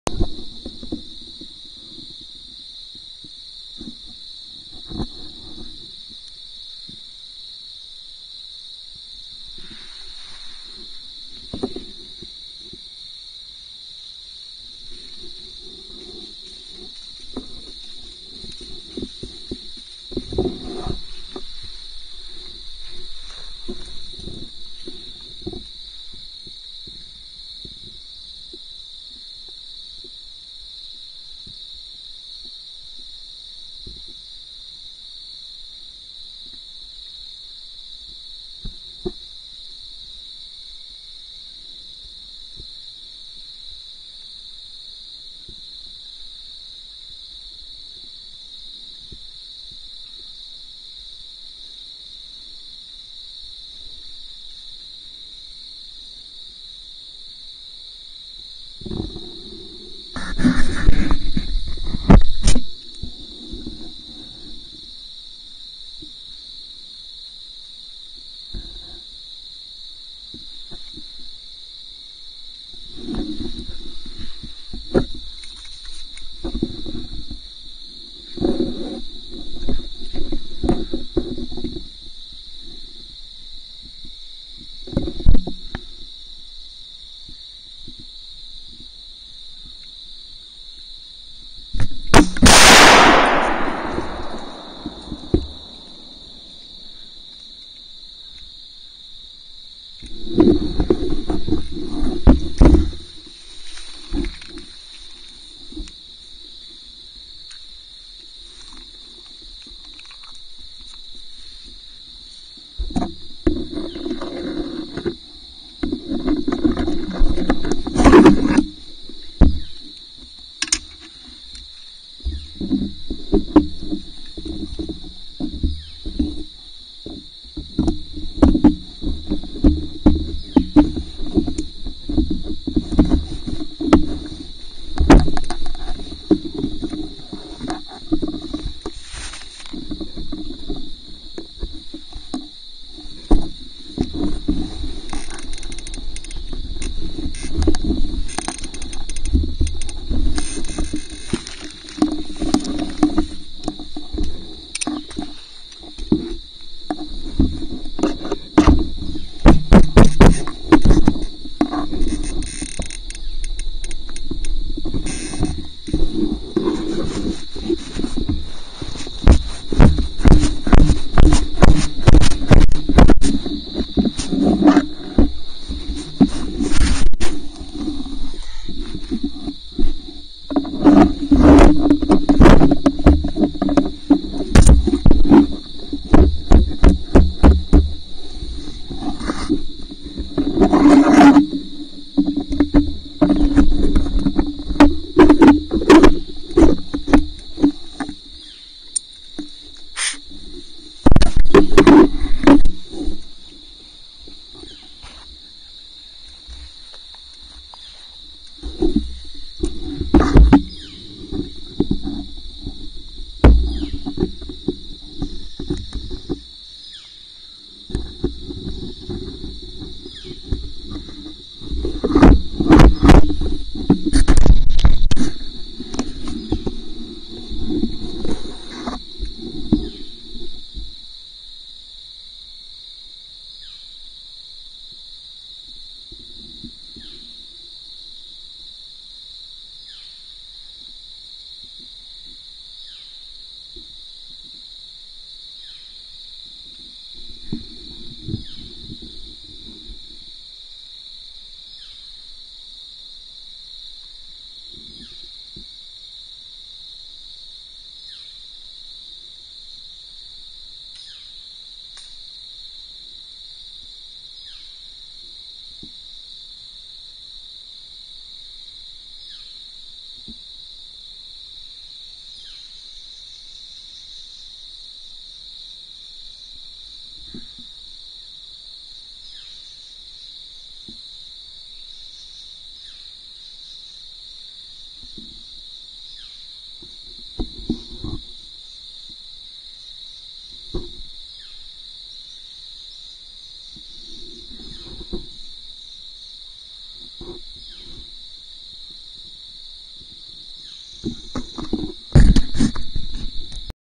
wma 2014 010
muzzle loader deayed and missed deer. First buck I seen. weather was hot.